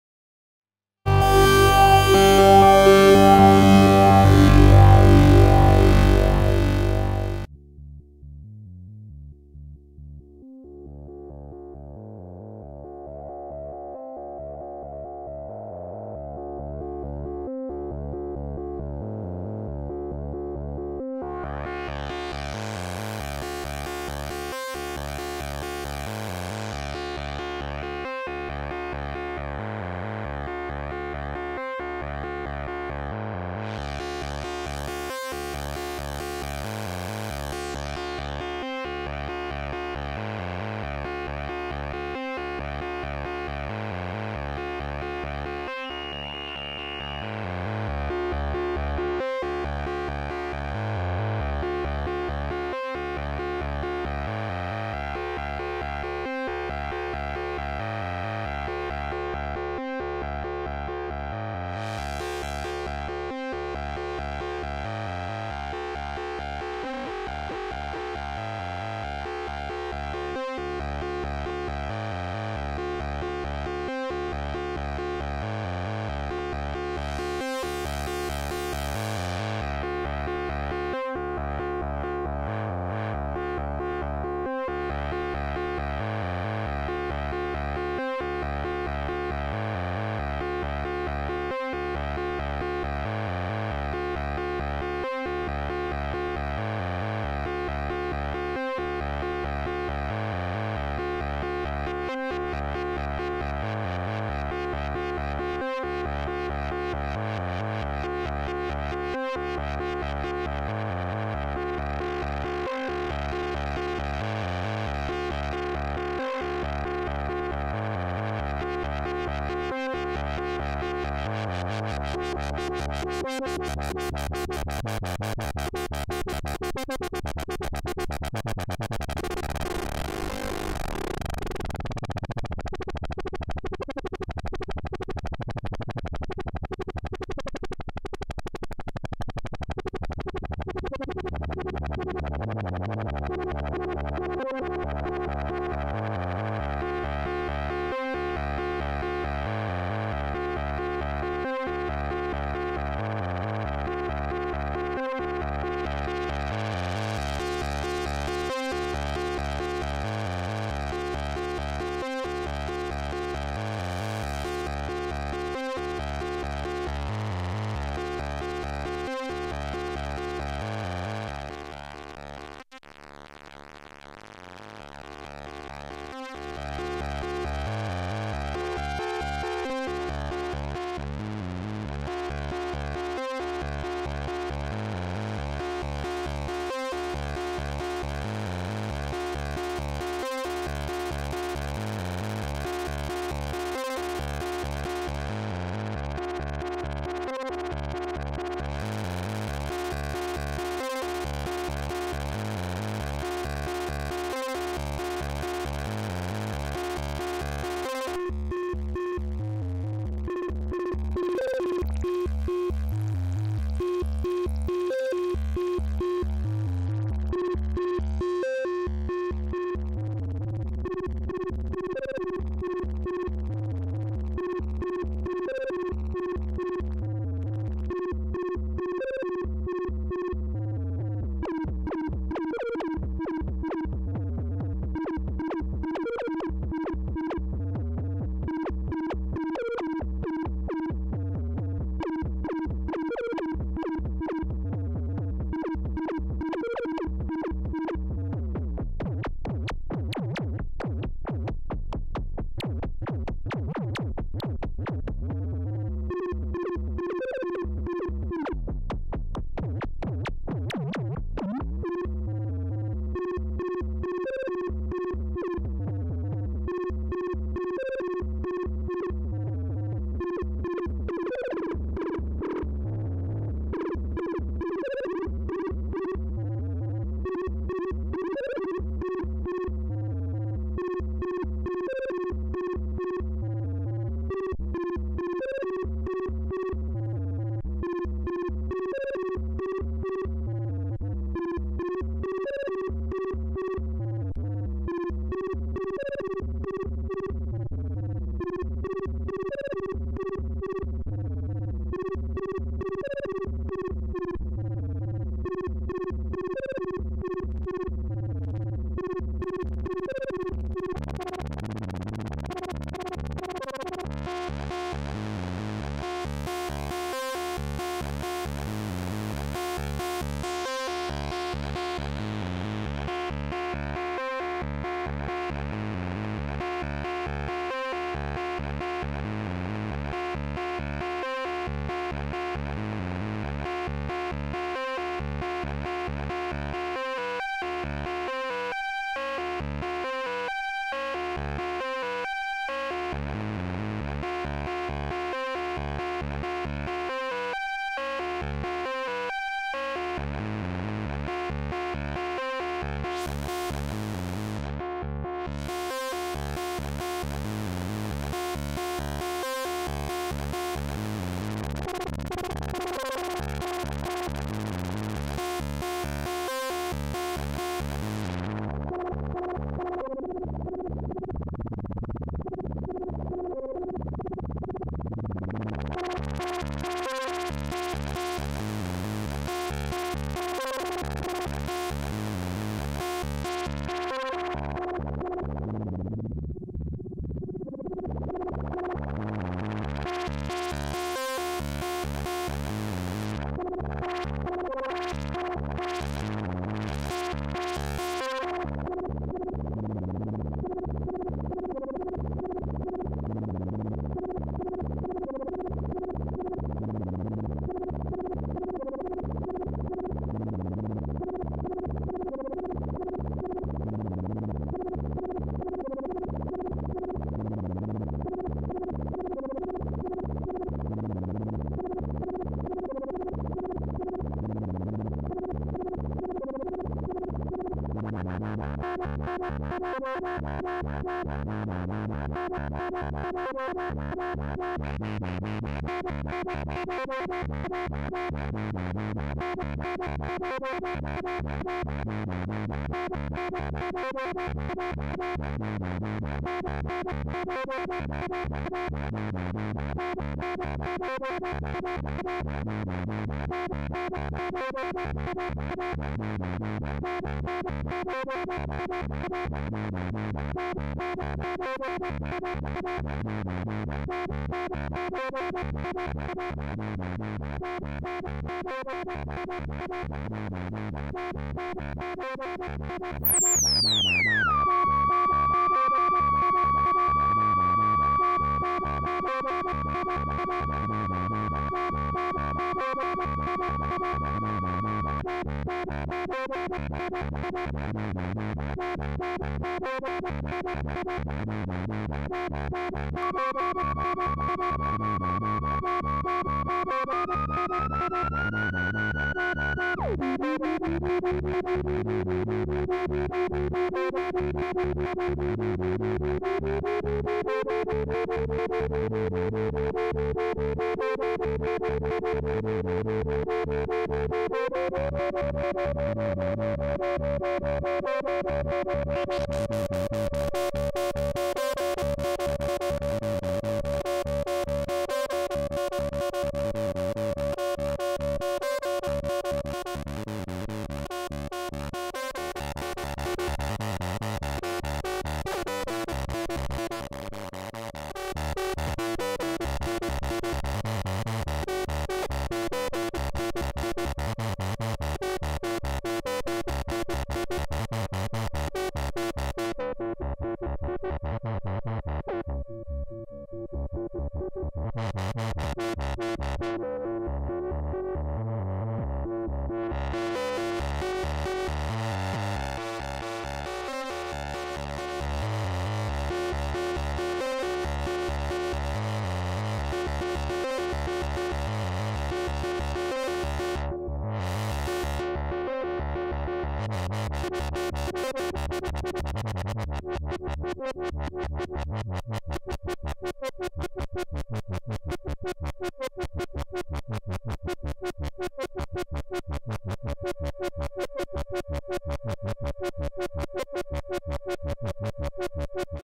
TIp Top Audio z8000 sequencer with Modular Synth
TipTop Audio sequencer, filter and oscillator, modulated by ASystems rs95e, rs85...